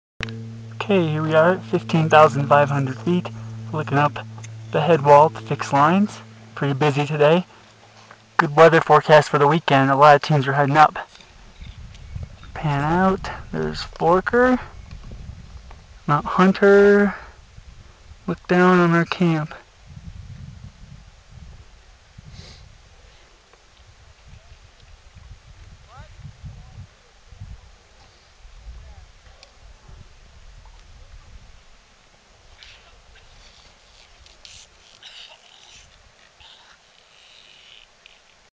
At the Fixed Ropes on Denali's West Buttress
Nearing the fixed ropes at 15,500 feet on Denali's West Buttress with a look back down to 14 camp